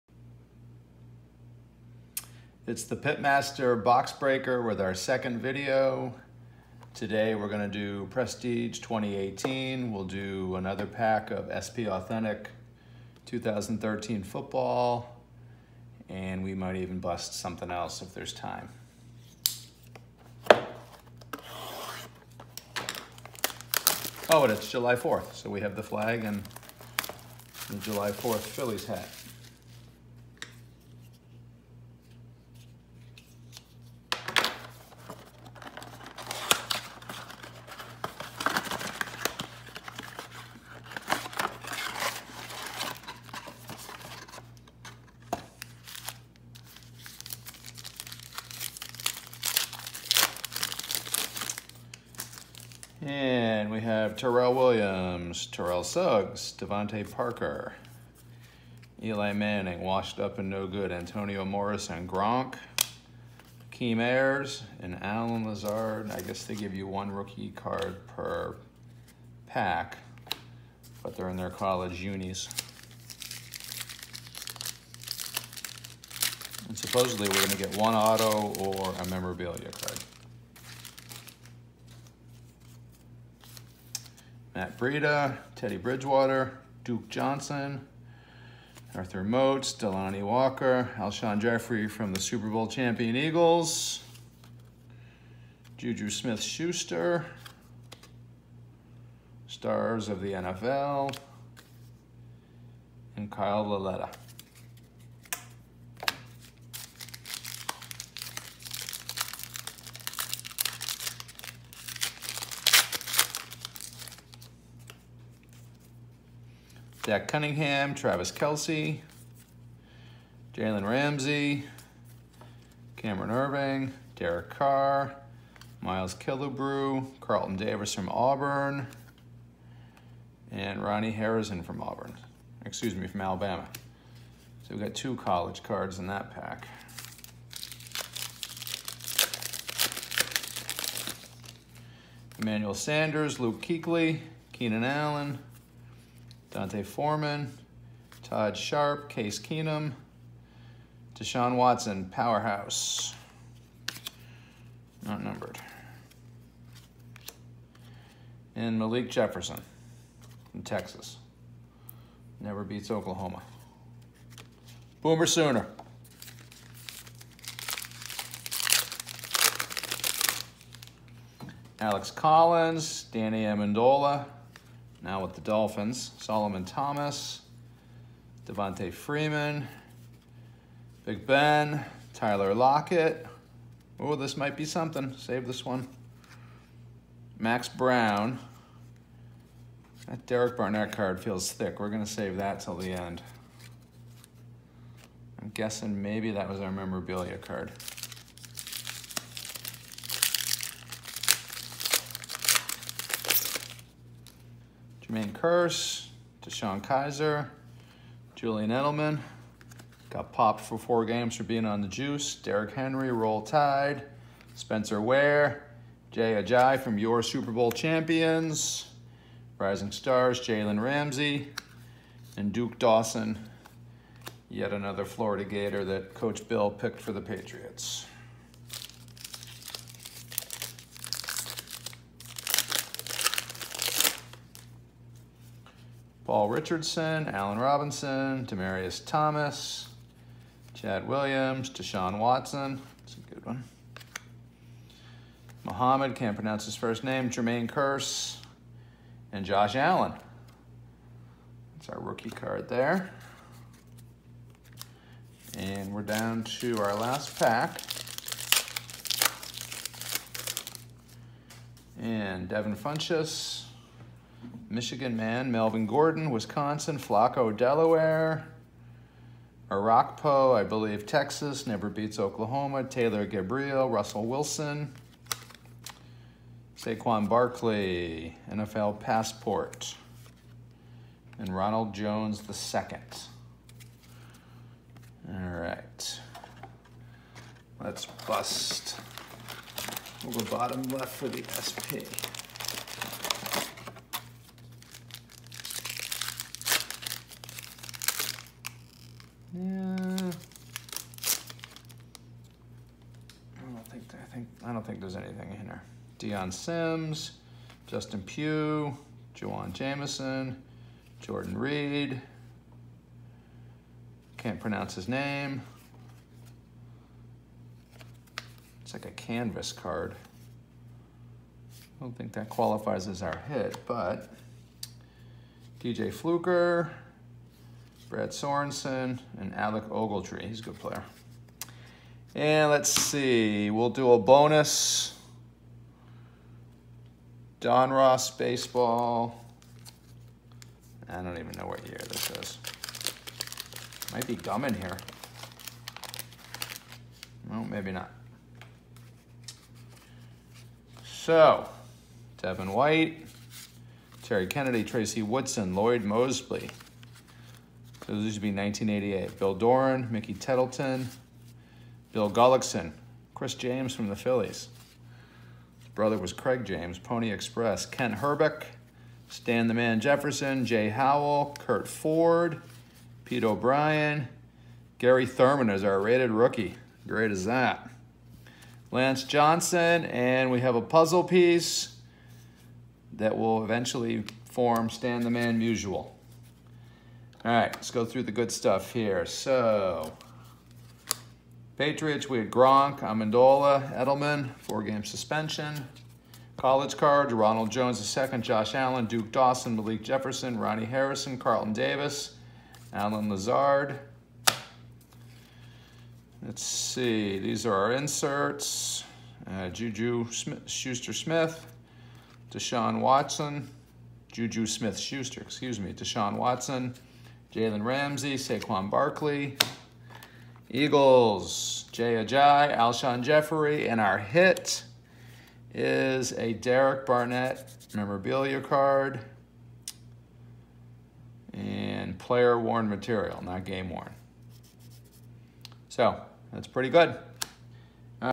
Second Box Break, July 4 🇺🇸
Prestige Football 2018 blaster, one pack SP Authentic 2013 Football, one pack 1988 Donruss Baseball.